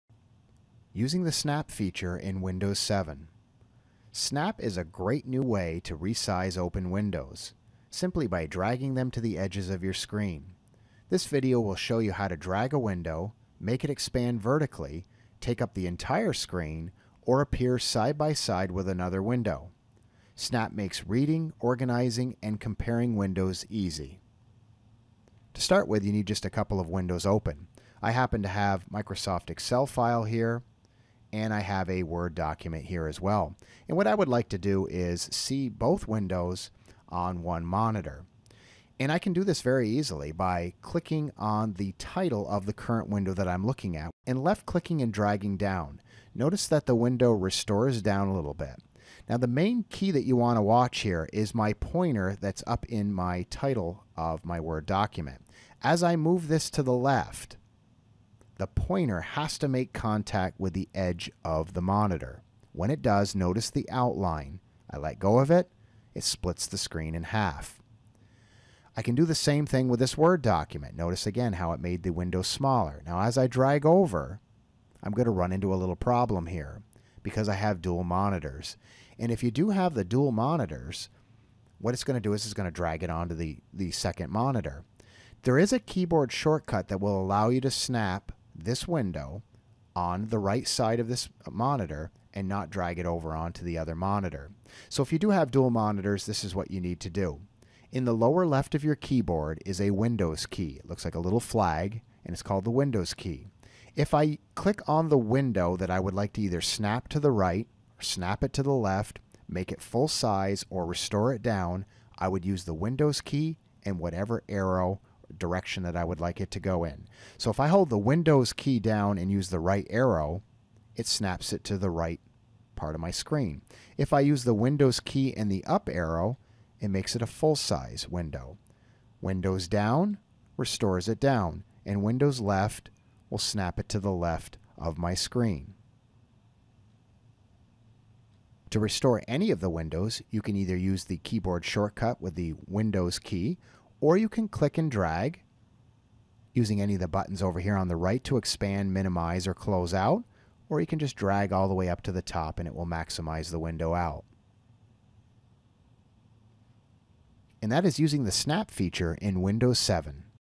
Using the Snap Feature in Windows 7
Snap is a great new way to resize open windows, simply by dragging them to the edges of your screen. This video will show you how to drag a window, make it expand vertically, take up the entire screen, or appear side-by-side with another window. Snap makes reading, organizing, and comparing windows easy.